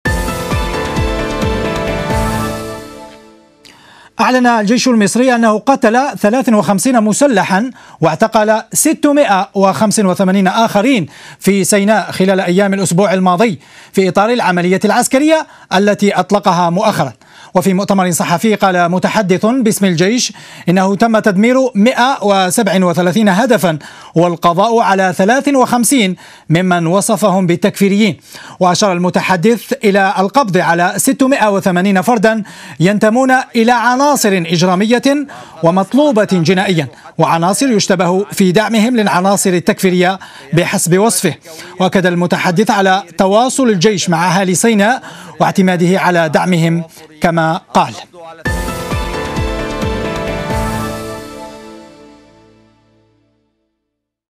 الجيش المصري يعلن عن "إنجازاته" في سيناء خلال أسبوع
أعلن الجيش المصري أنه قتل 53 مسلحا واعتقل 685 آخرين في سيناء خلال أيام الأسبوع الماضي، في إطار العملية العسكرية التي أطلقها مؤخرا.

وفي مؤتمر صحفي، قال متحدث باسم الجيش إنه تم تدمير 137 هدفا، والقضاء على 53 ممن وصفهم بـ"التكفيريين".